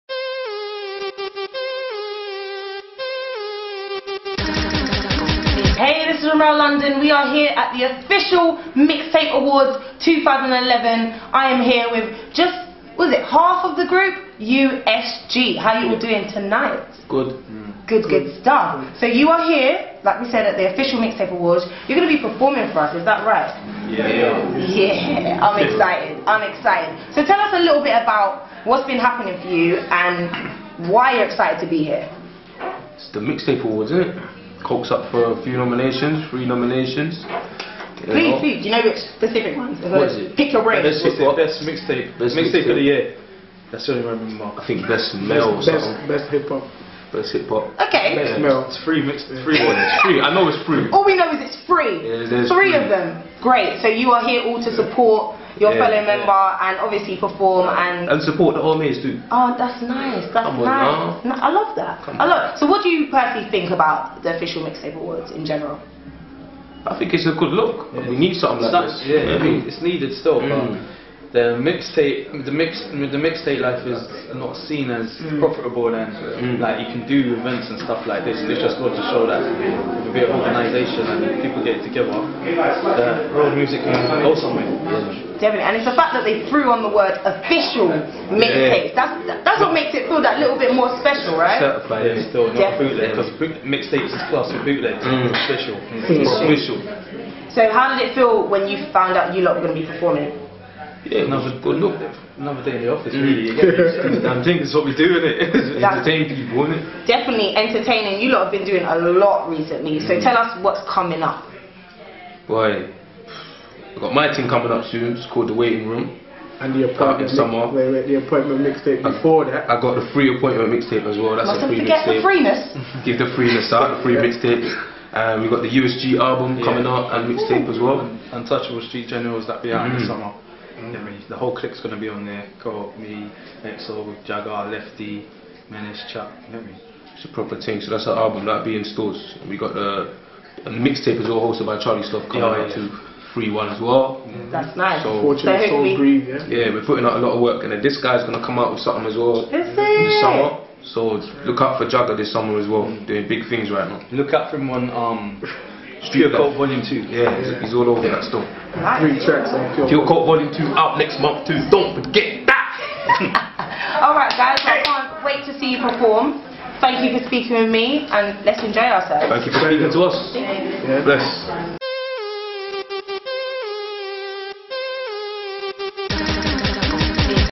USG Back Stage @ The Official OMA's 2011 Interview.
Highlighs from the Official OMA Award show, Official Mixtape Awards 2011 Look out for the Official Mixtape Awards 2012 Coming Soon